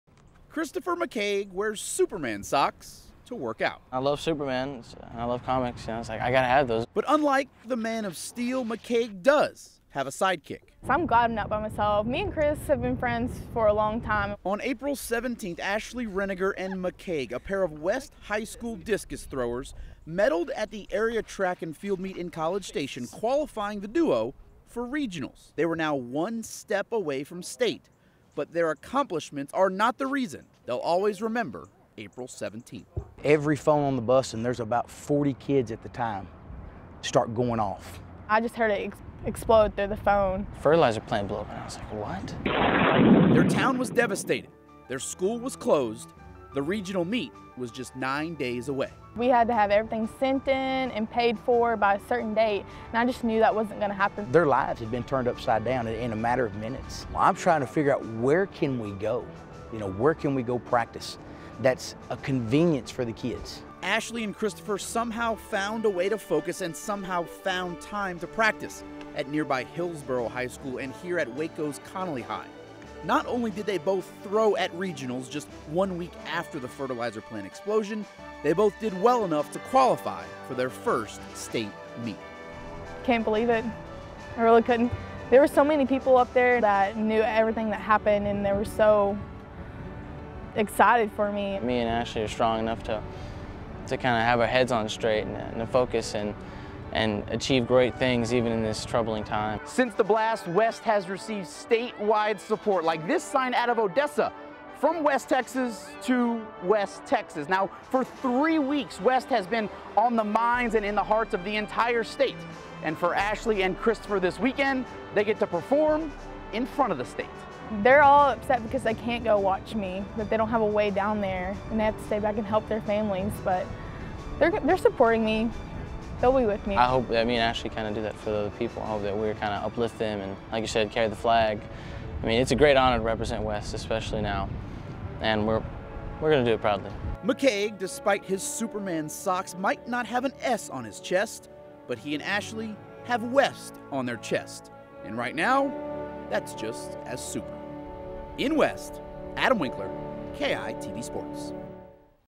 "West Does Not Rest" by Adam Winkler on 5/9/13
A pair of West High School students did not allow the disaster in their hometown end their athletic careers. Now, Ashley & Christopher are flying the flag for West, Texas on the state's biggest stage.